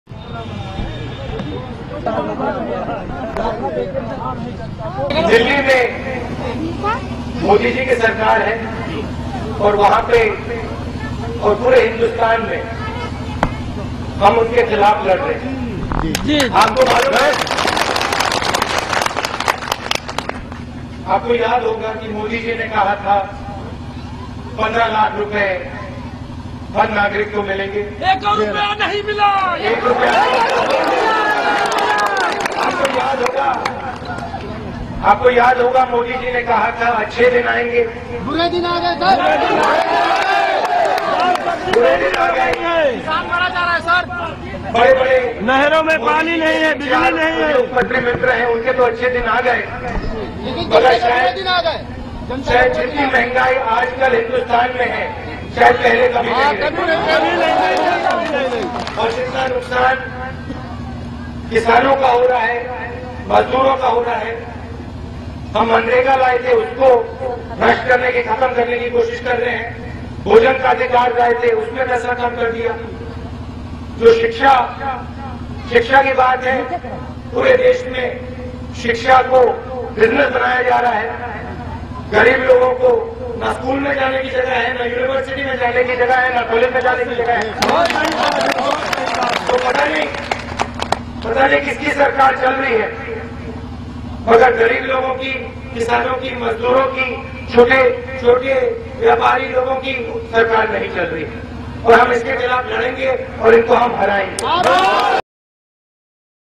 राहुल गांधी ने साधा नरेन्द्र मोदी पर निशाना
कहा-पूरे देश में हम केन्द्र सरकार के खिलाफ लड़ रहे हैं
कहा था, अच्छे दिन आएंगे मगर बुरे दिन आ गए देश में महंगाई और बढ़ गई